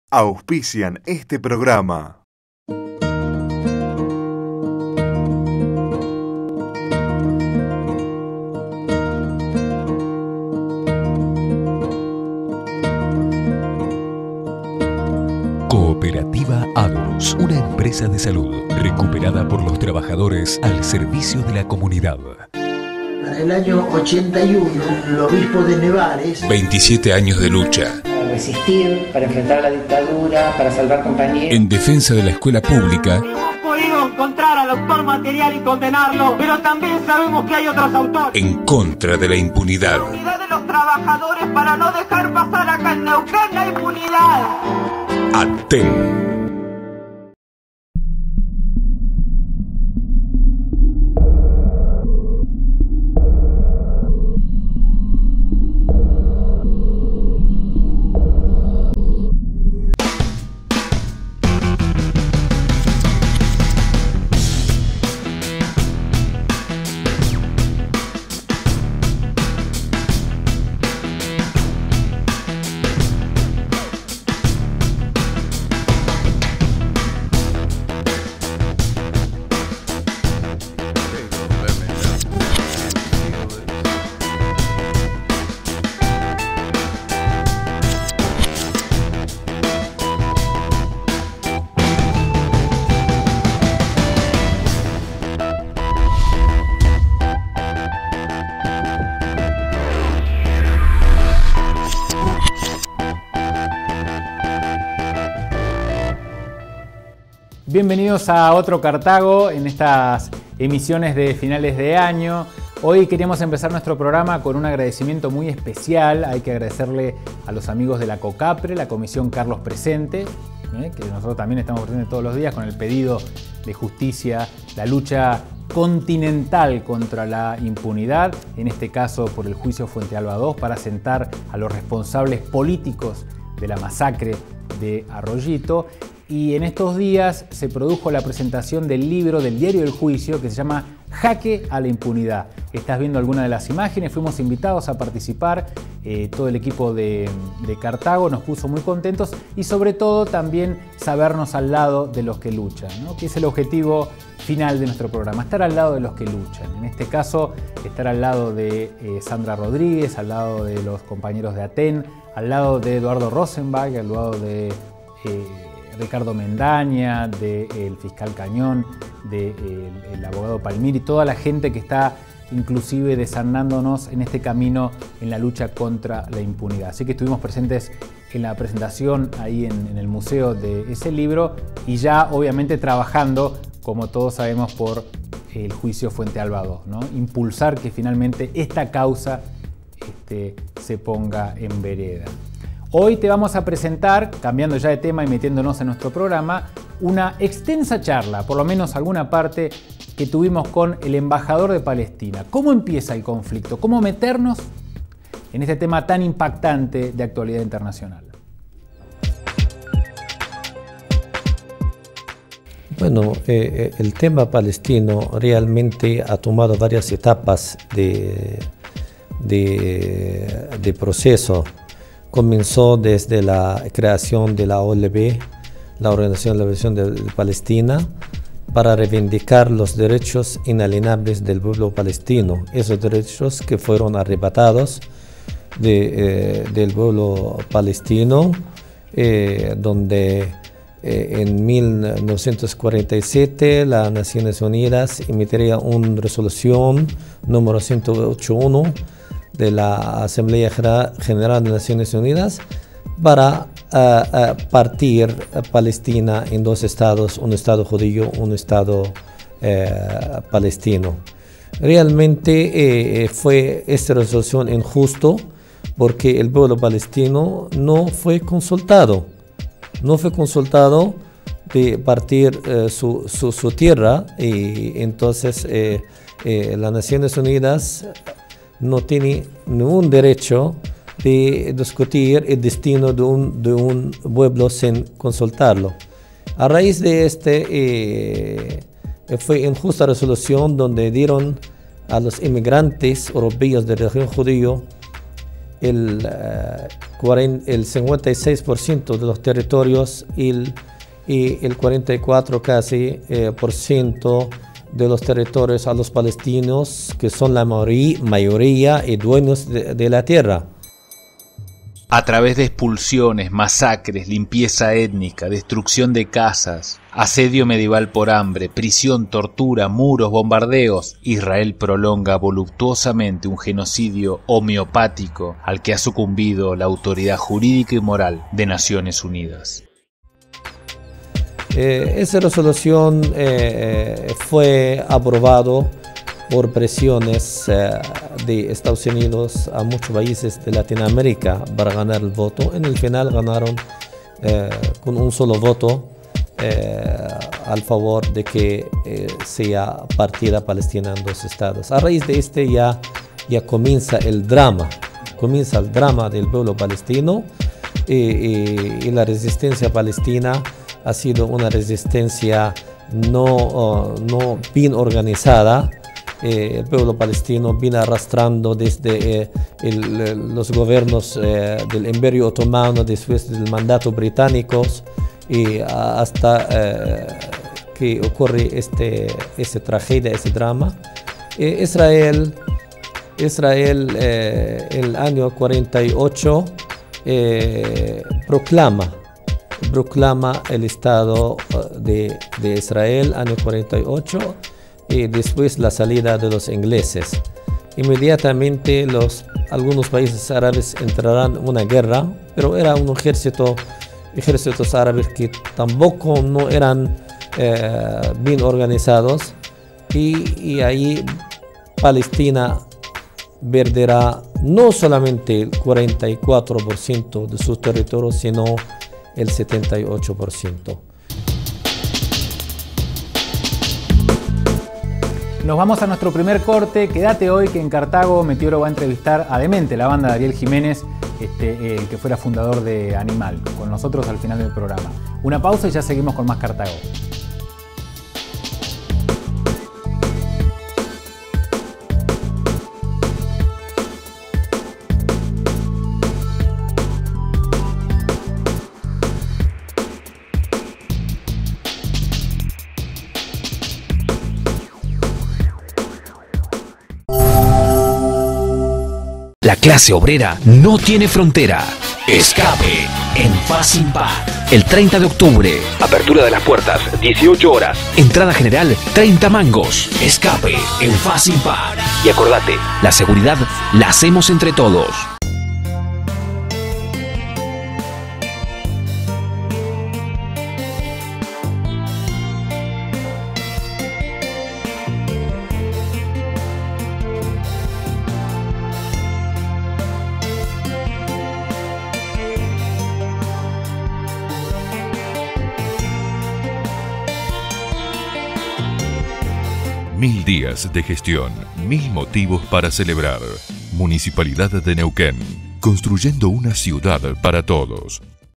CARTAGO 2010 PALESTINA VENCERA (I)
Primera parte de la charla con el Embajador de Palestina, Walid Muaqqat, quien relata los origenes de la tragedia del Pueblo Palestino.